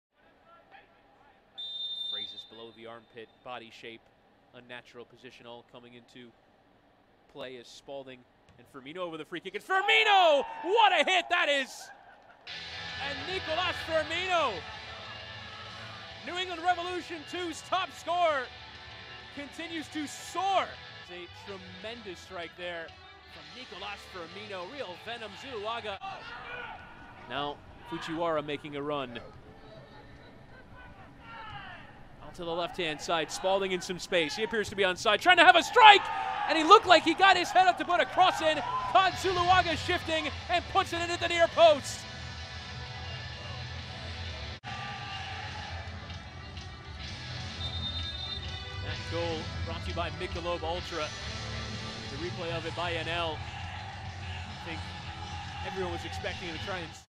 League One Player of the Week - Nicolas Firmino, New England Revolution II
USL League One announced the Team of the Week Presented by Cleer Audio for Week 13 of the 2020 regular season on Tuesday, with the New England Revolution II’s Nicolas Firmino earning Player of the Week honors after leading his side to a fourth consecutive victory on Friday night as the Revolution II downed Fort Lauderdale CF.